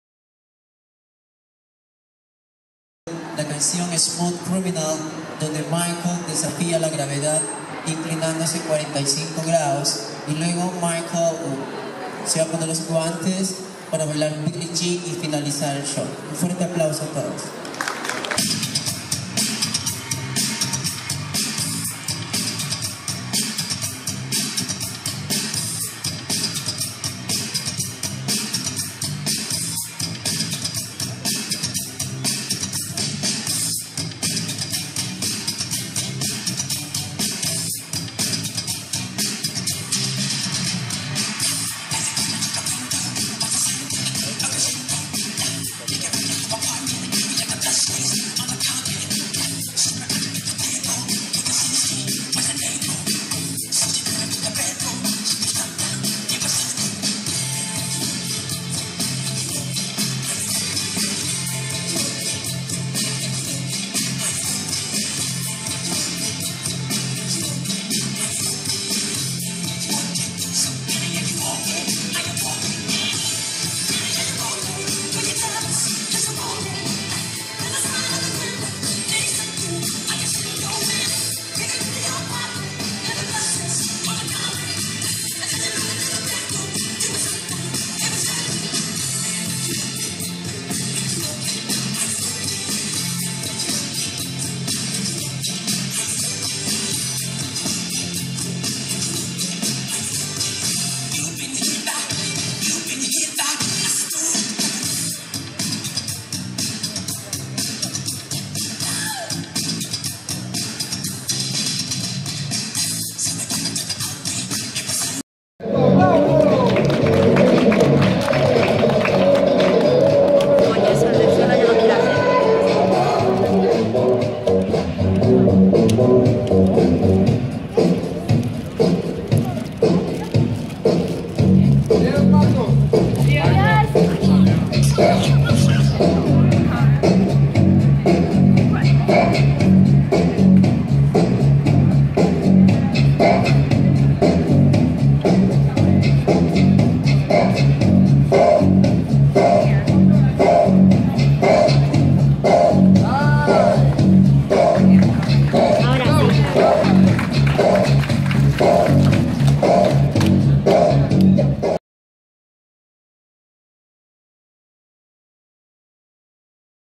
MICHAEL JACKSON RESSUSCITE OU IL N'EST PAS MORT
Partout dans ce monde on trouvent des Jacksons ou presque par la danse la chanson ou même physiquement